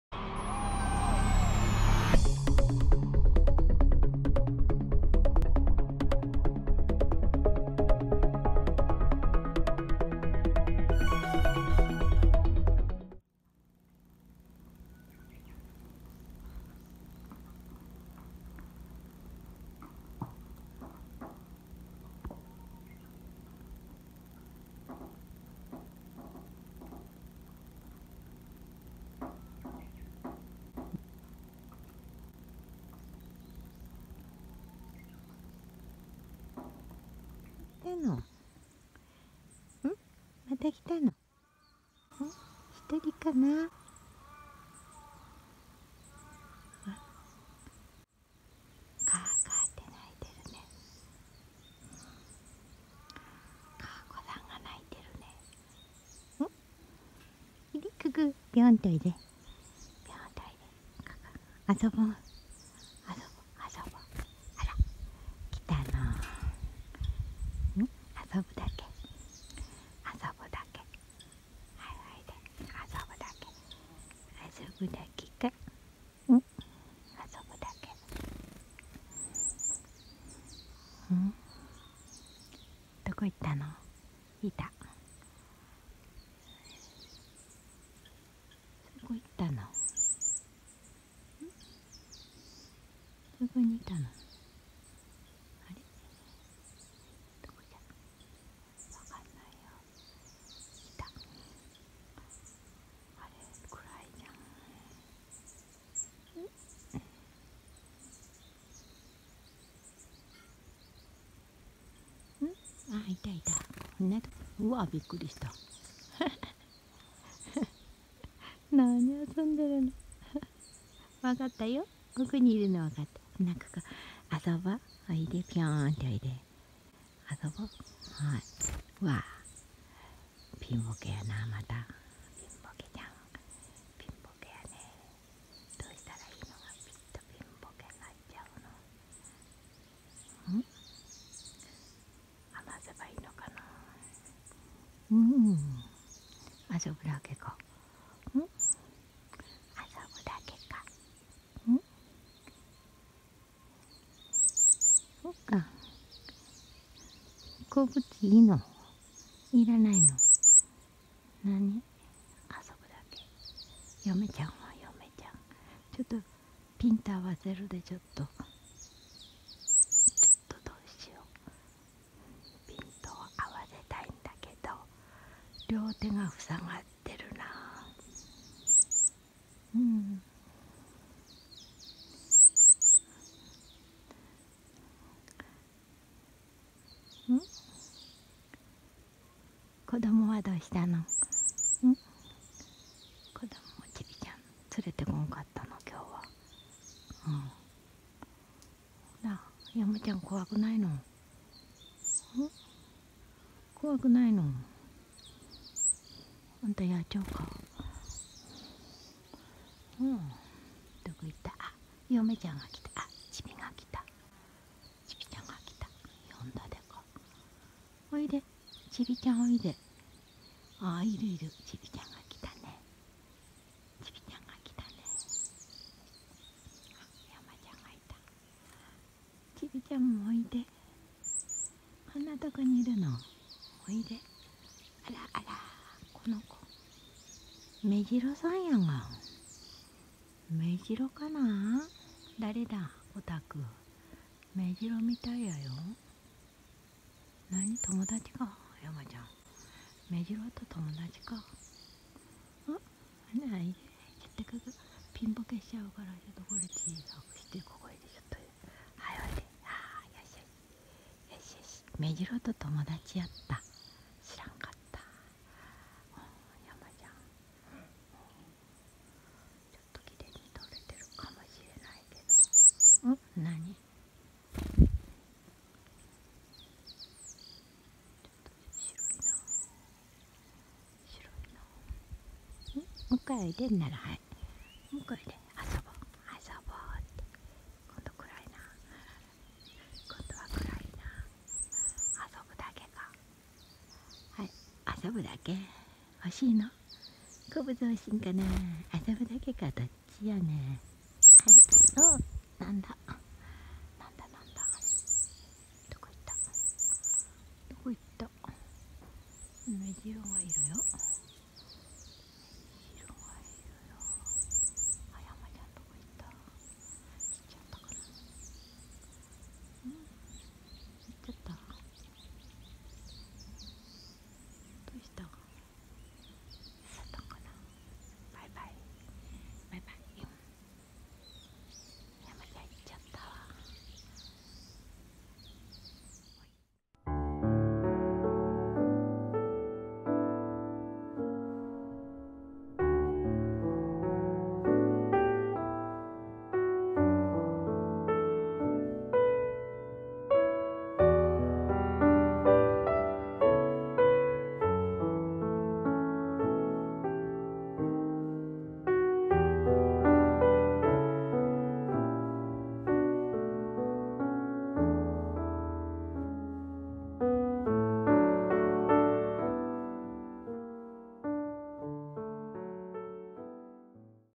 ヤマガラは窓ガラスをコンコン叩いて何がしたいのか？人間を庭に誘い出し何がしたいのか？
子育て中のヤマガラのお父さん（ヤマちゃん）は、夕暮れになると、窓辺に誘引したバラの枝によじ登りクチバシで窓ガラスをコンコンとたたき、来たことを知らせます。
鳴き声を出せば敵に巣箱の場所がわかり、ヒナが狙われてしまいます。
ヤマちゃんは鳴き声で来たことを知らせずに、ガラスをたたき庭に出てくるように誘いました。
庭に出ると、いつもの場所の椅子の背もたれに止まって待っていました^^

ヤマガラは春と夏、年に２回子育てをします。
一日中、何度もなんども巣箱にエサを運び、夕方にはもうクタクタになって疲れ果てた〜！と言った感じです。

ホッと息抜きをして、疲れを癒して欲しいのでしょうか〜？
窓ガラスをコンコン叩いて庭に呼びだし、桜の枝と私の指を行ったり来たりして遊ぶのです。

☆庭に遊びにくる野鳥のBlog「とりのる」も覗いてみてください！